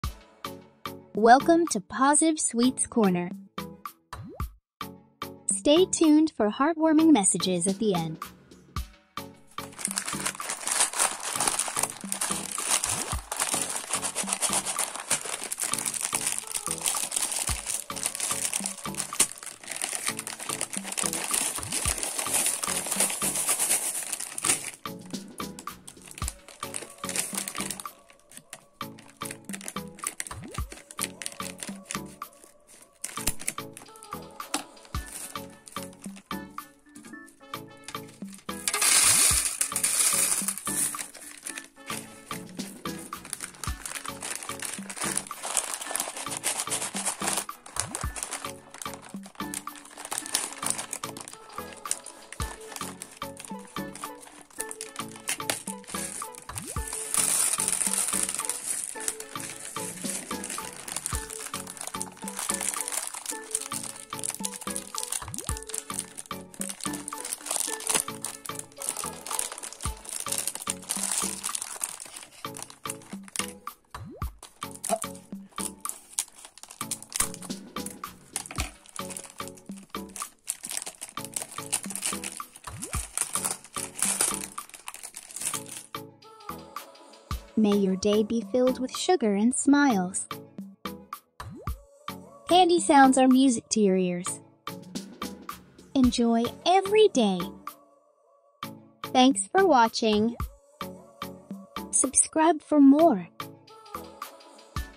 ASMR unpacking sweets. M&M's. Choco Mint.❤️
Hi! In this video,  I opened two different packs of M&M's and one pack of Choco Mint.
In addition,  there is a positive message at the end of the video for both sweet lovers and ASMR lovers. I now try to post everyday videos like this with different sweets and messages.